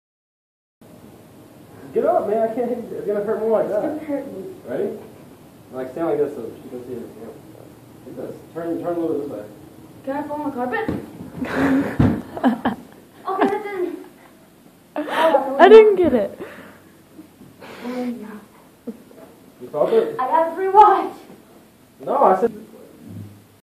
Proof that my brothers stronger than yours
He wanted one of my watches so I let him have it if he could take a hit.. I let him use a pillow